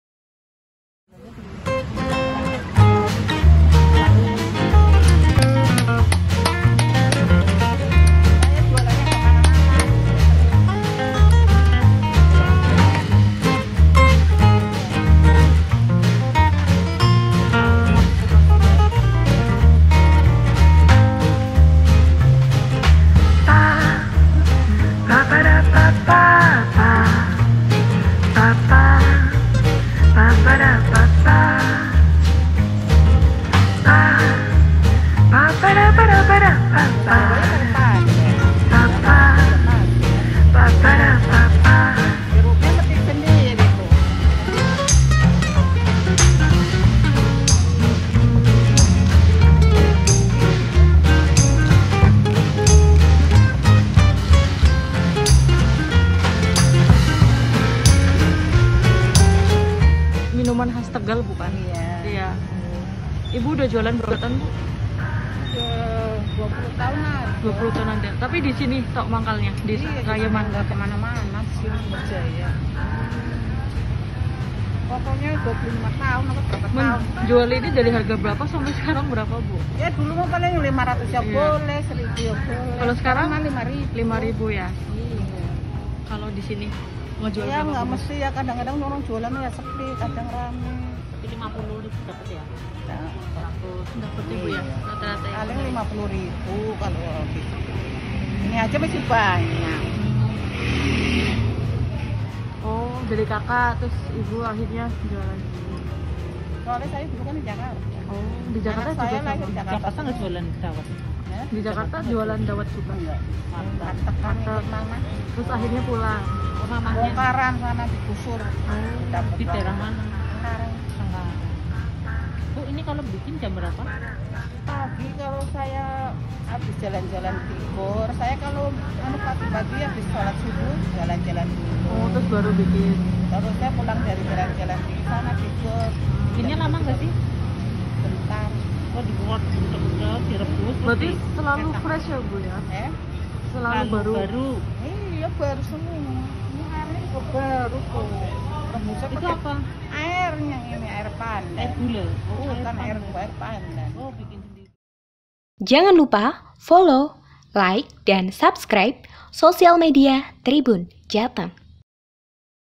Segarnya Dawet Beras Khas Tegal Bu Dariah Cuma Rp 5 000
Ketika Kab Banjarnegara memiliki minuman khas dawet ayu, maka di Tegal juga ada yaitu dawet beras yang cita rasanya berbeda. Dari banyaknya penjual dawet beras khususnya di wilayah Kab Tegal, satu yang masih eksis dan bertahan sampai sekarang yaitu bu Dariah yang biasa mangkal di depan Toko mas Sumber Jaya, Pasar Trayeman Slawi.